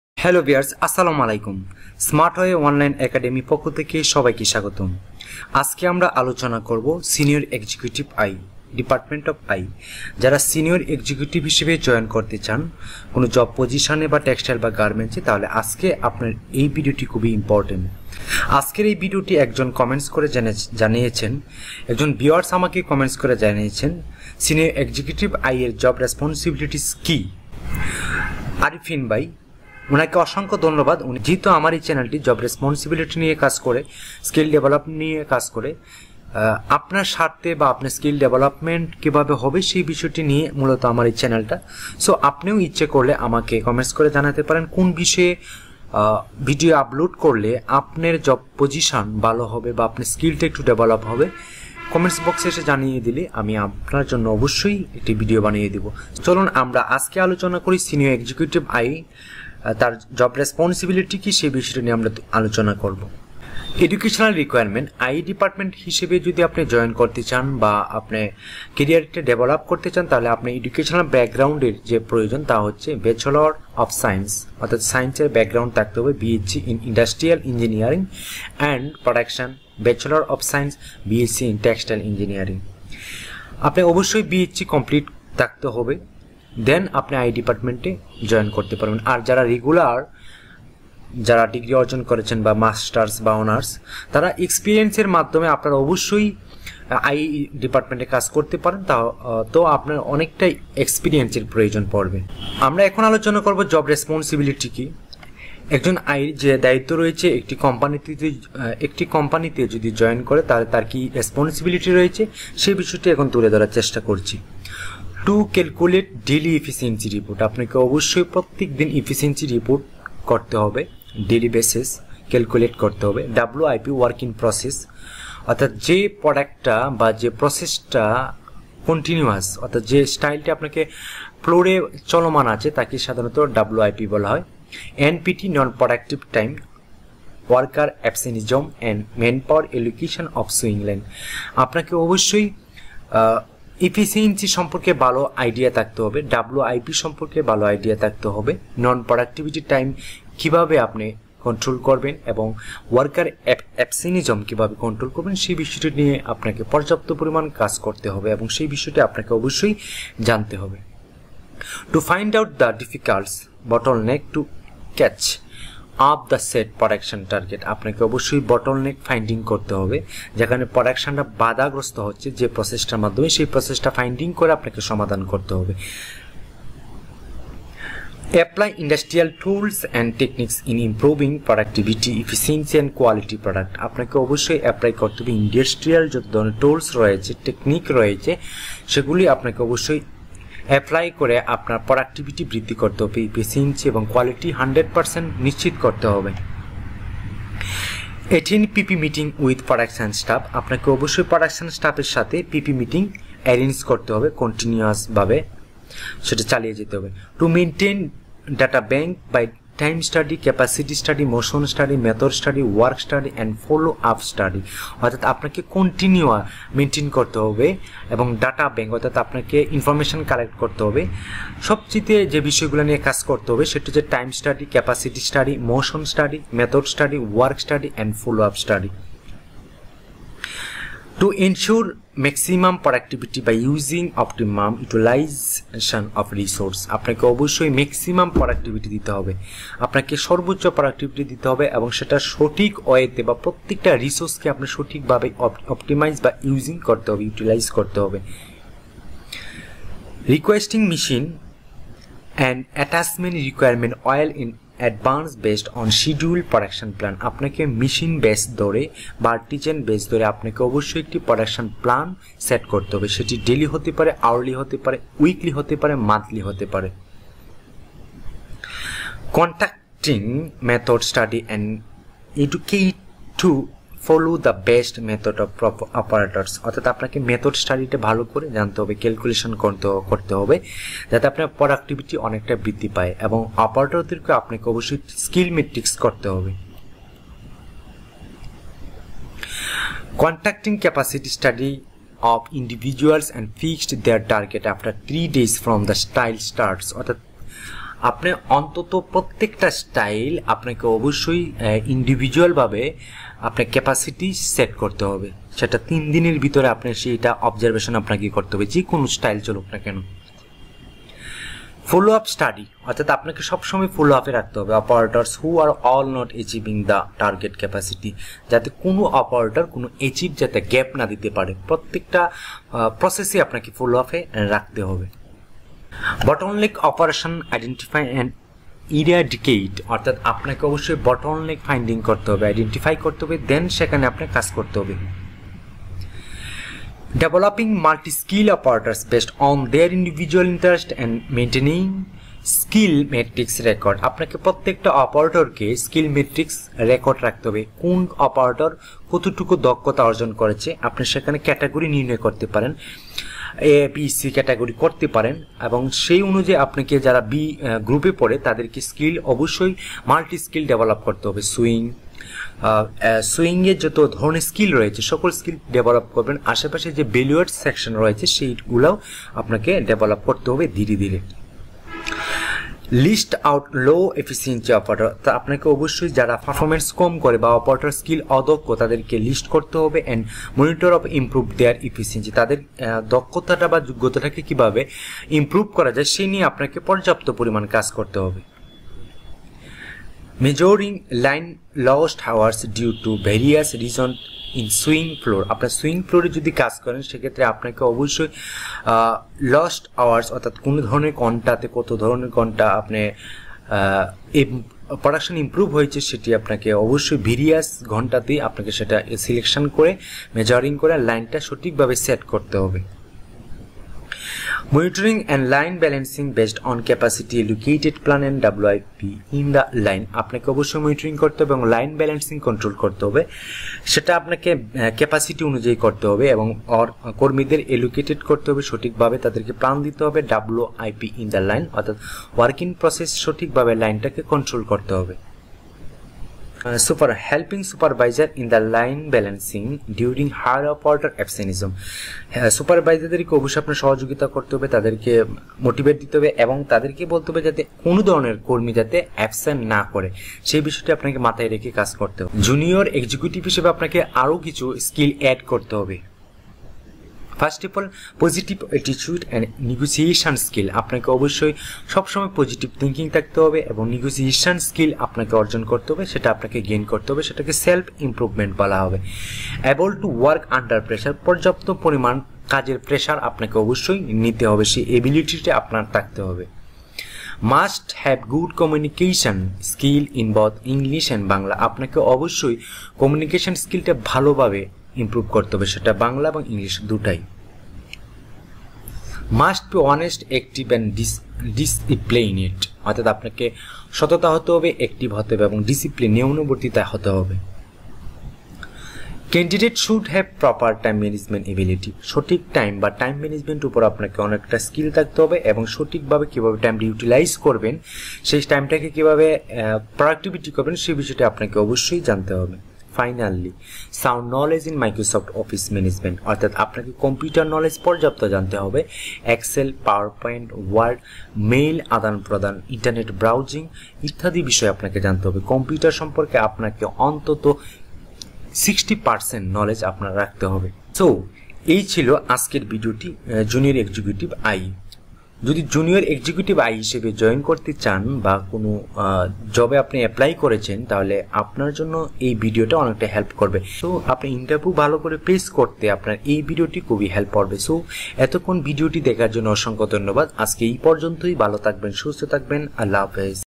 Jr Executive IE । Job Responsibilities । Development of IE
Today I will discuss Jr. Executive IE । Job Responsibilities । Development of IE